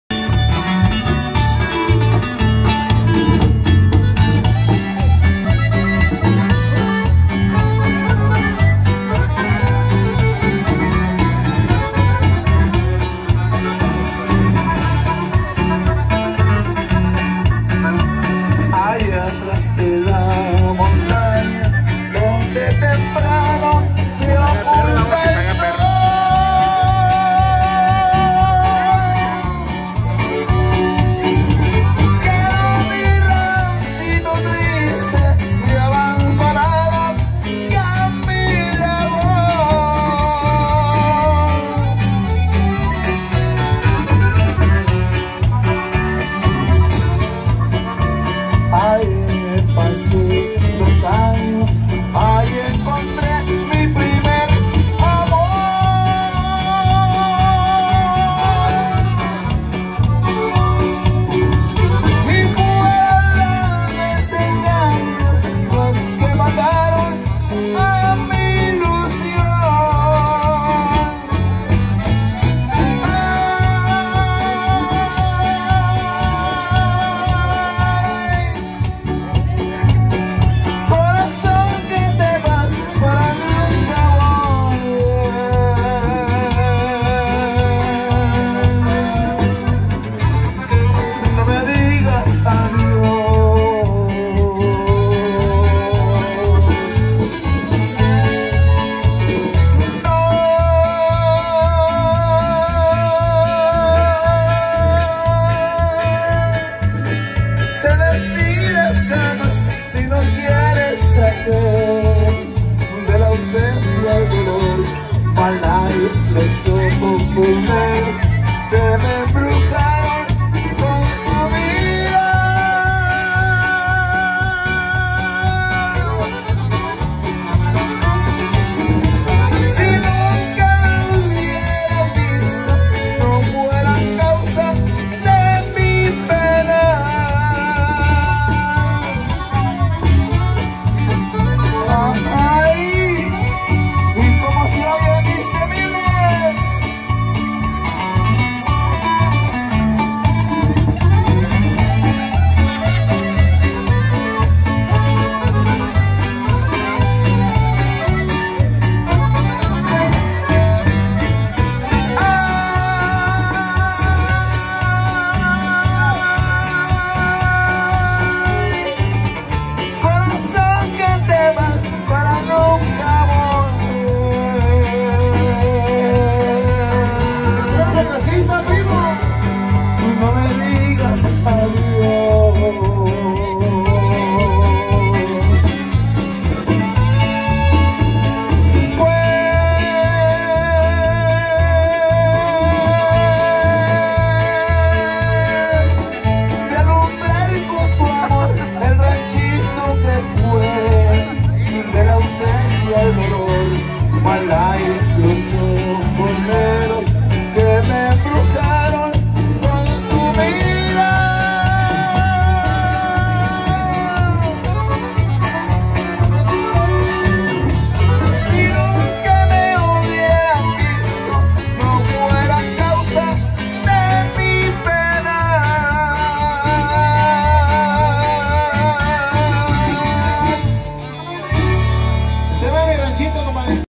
Los Temibles / Rancho la Casita, Chihuahua/ Mi Ranchito / March 20 2021
15 Años en la Casita con los Temibles de Chihuahua, 20 de Marzo de 2021.